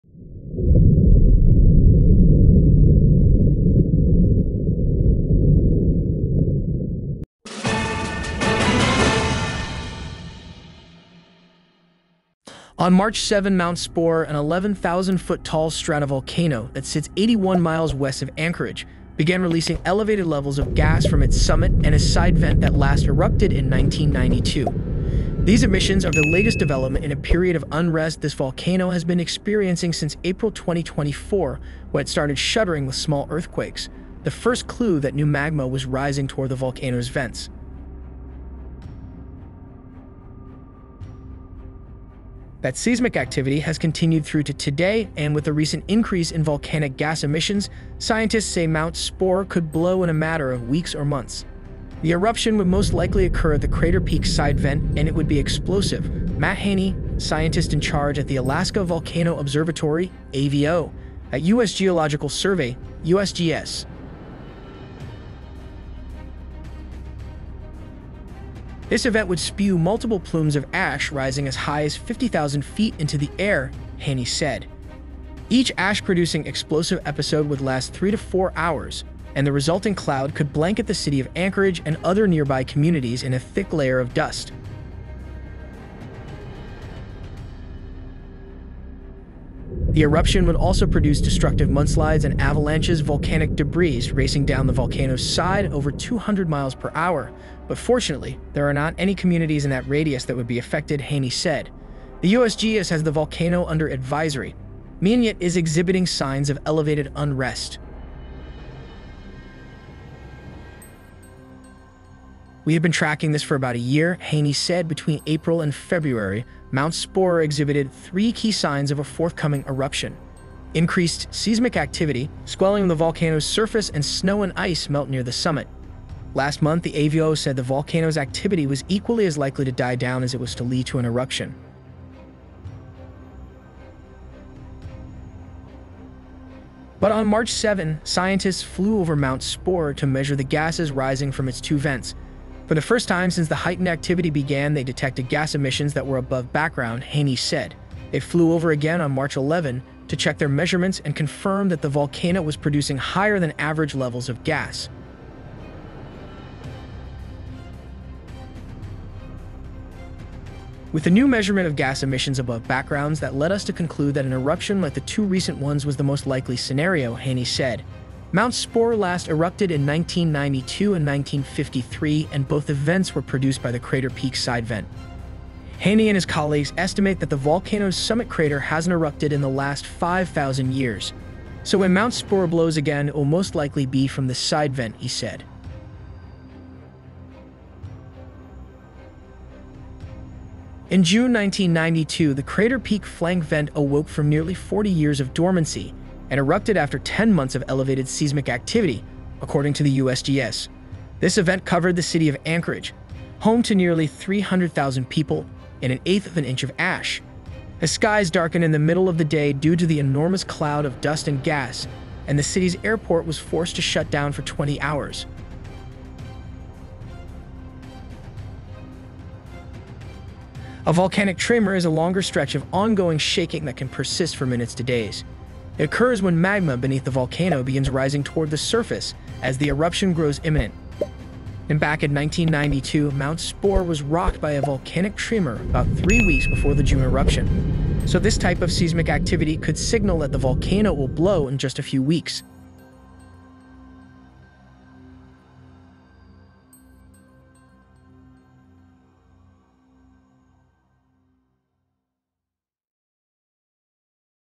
Alaska Alert! Emergency Preparations for Mount Spurr Eruption, Volcanic Ash in Anchorage
Alaska Alert! Emergency Preparations for# Eruption, Volcanic Ash in Anchorage

On March 7, Mount Spurr, an 11,000-foot-tall stratovolcano that sits 81 miles west of Anchorage, began releasing elevated levels of gas from its summit and a side vent that last erupted in 1992.  These emissions are the latest development in a period of unrest this volcano has been experiencing since April 2024, when it started shuddering with small earthquakes — the first clue that new magma was rising toward the volcano's vents.